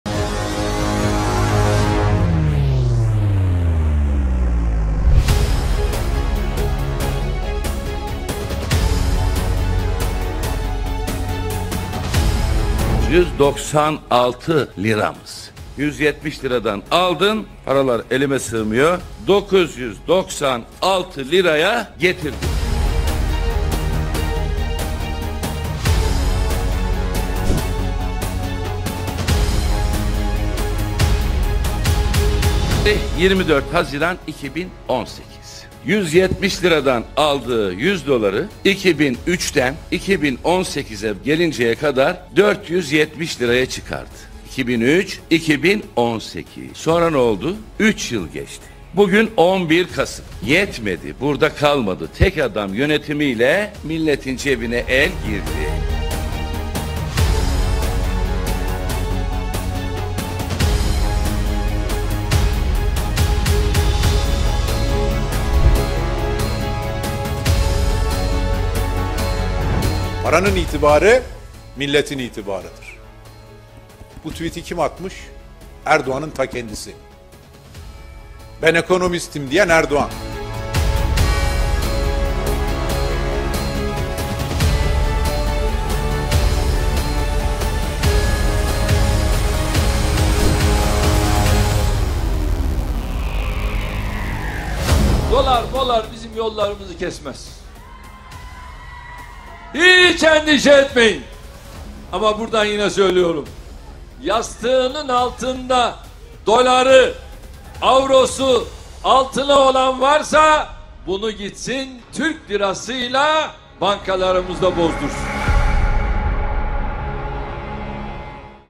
المعارضة التركية عن انهيار العملة: أردوغان يضع يده في جيوب الأتراك
المعارضة التركية تحمل أردوغان مسؤولية انهيار العملة ومد يده في جيوب الأمة: كنا نشتري الـ100 دولار بـ170 ليرة واليوم نشتريهم بـ996